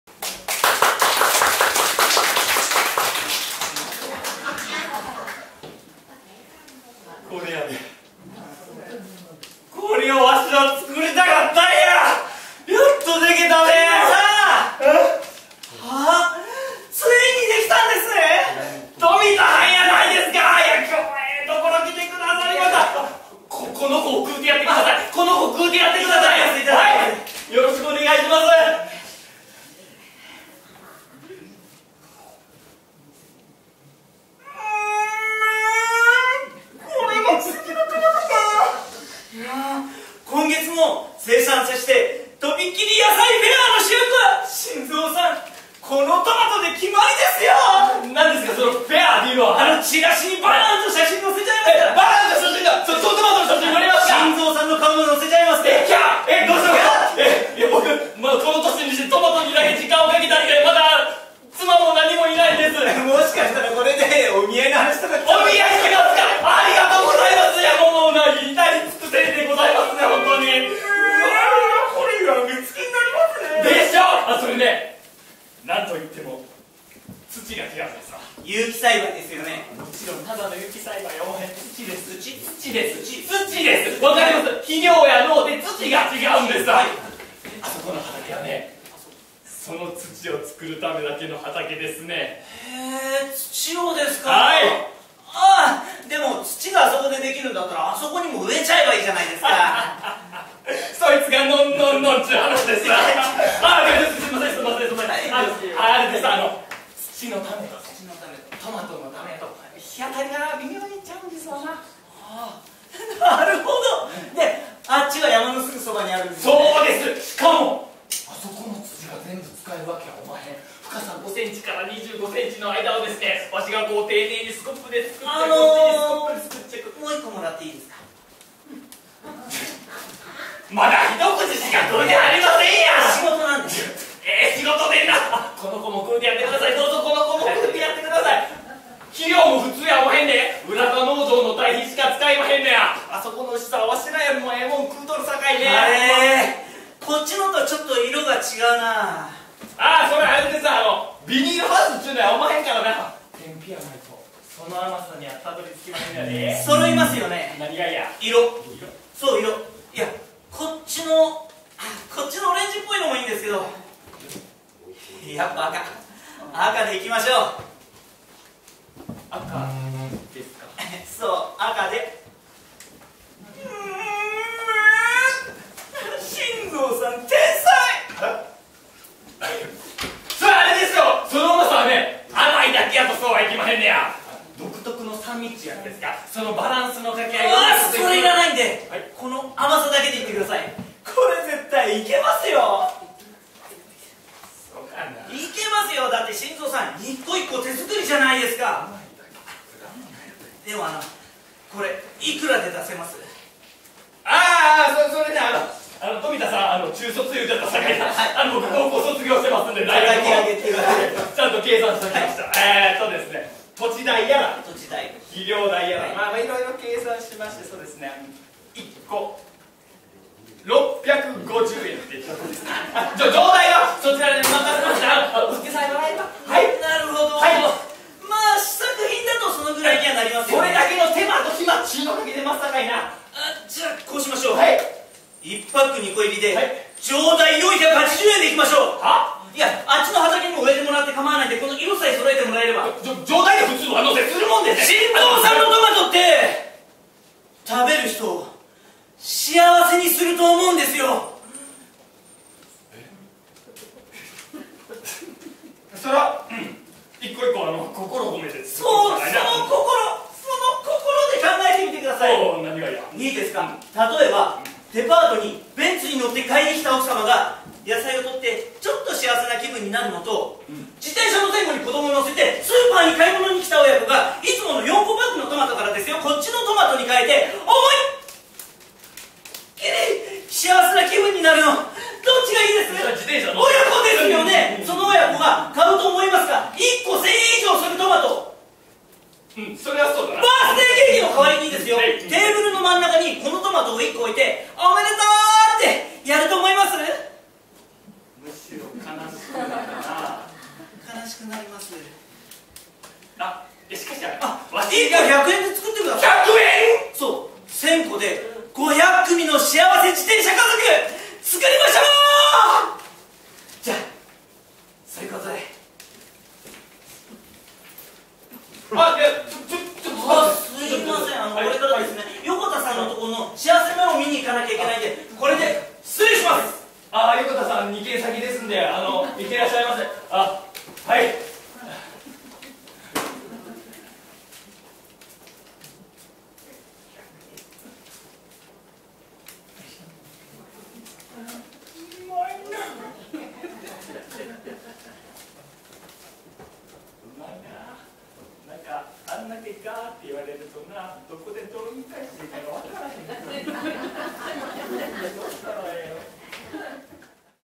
モクレン2011『トマト物語』＠福島県いわき市・双葉町仮設住宅
モクレン2011『トマト物語』2012,7,27　福島県いわき市・双葉町仮設住宅にての慰問公演
出演：米倉啓、あまち　作／演出：ふるいちやすし